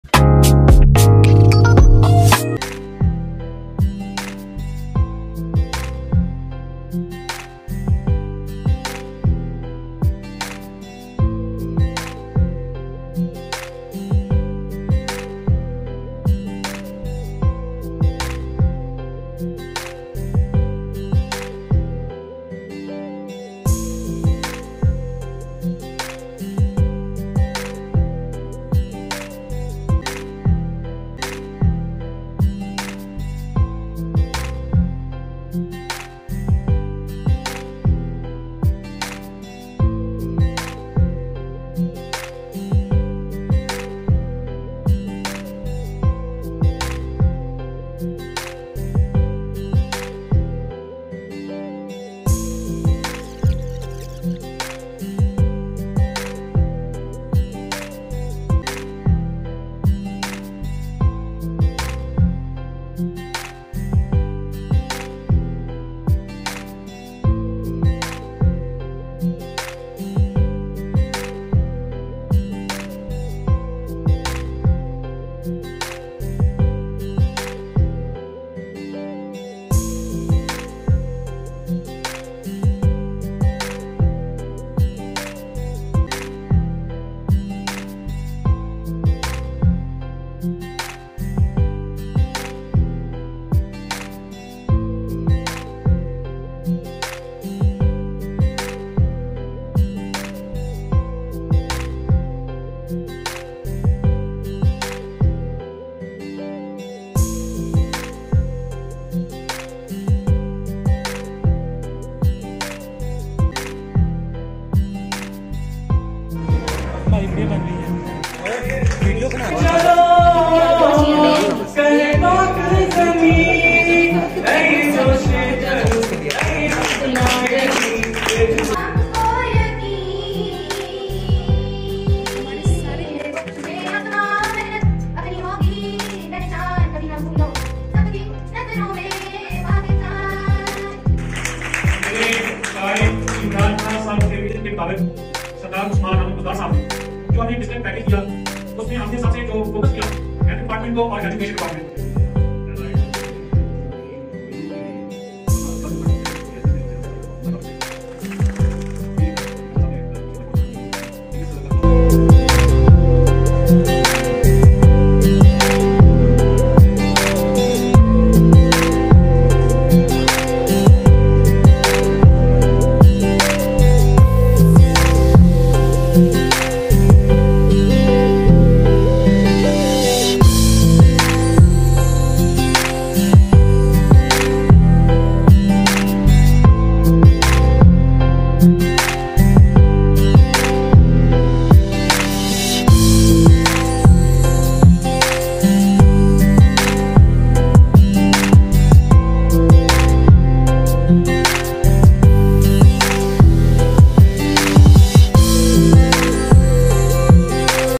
Annual Prize Distribution ceremony 2021 | Matric Result | Divisional Public school | Vlog #2 ✨❤️
Assalam-o-Alaikum!
MAKE SURE TO SET THE QUALITY OF VEDIO AT 1080p
Hope you guys enjoy my second vlog ! 🙂✨❤️🤝